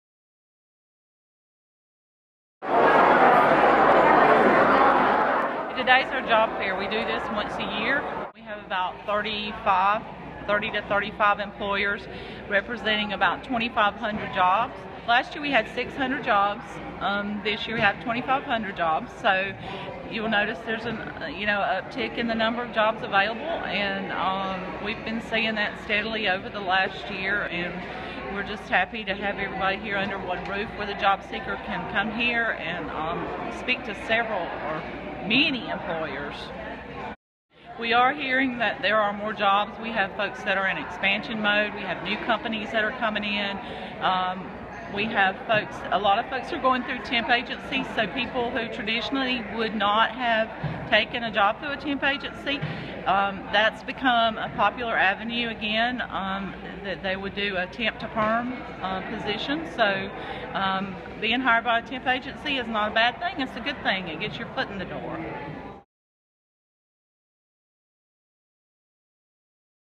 SC job fair in Chester County
Hundreds came out Wednesday for the S.C. Works job fair at the Gateway Conference Center in Richburg.